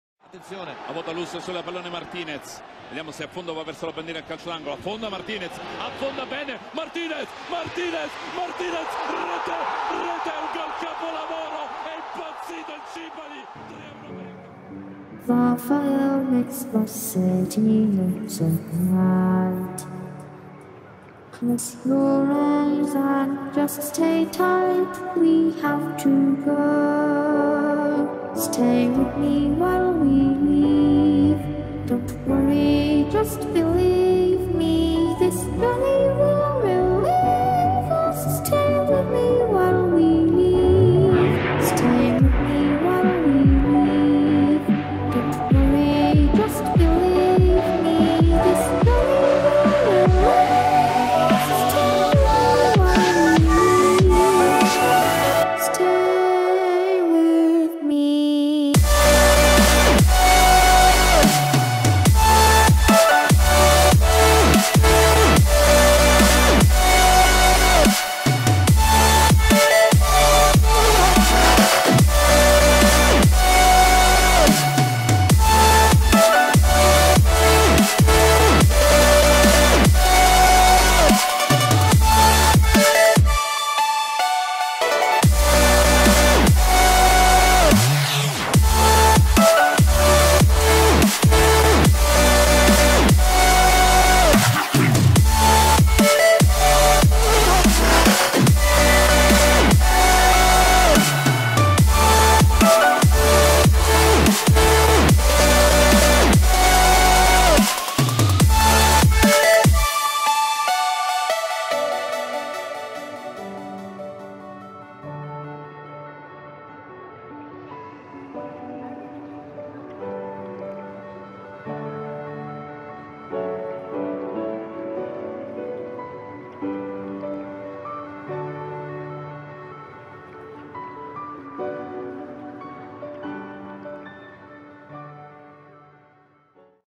Goodbye Jorge Martinez || El Malaka
One of the best and magic football player of every time go away from the played Football!!! Goodbye Jorge ! You will be forever the only Malaka! (Malachia)